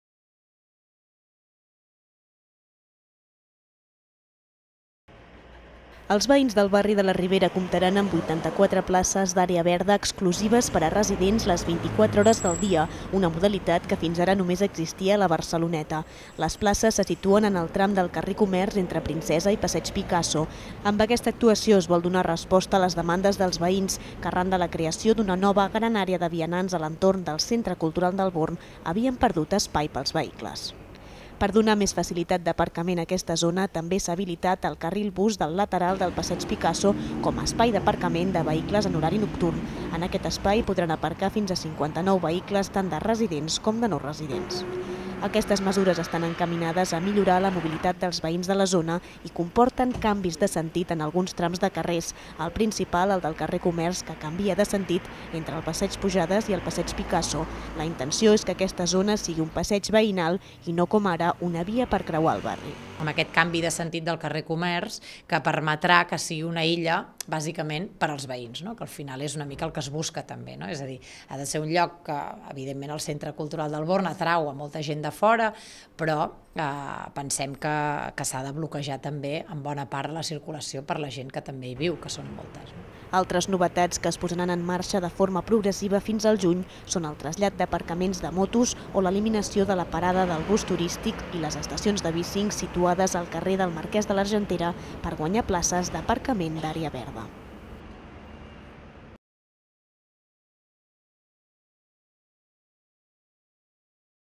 Els veïns del Born guanyen 84 places d'aparcament exclusives per a residents
Amb aquesta actuació es vol donar resposta a les demandes dels veïns que arran de creació d'una nova gran àrea de vianants a l'entorn del Centre Cultural del Born havien perdut espai per als vehicles.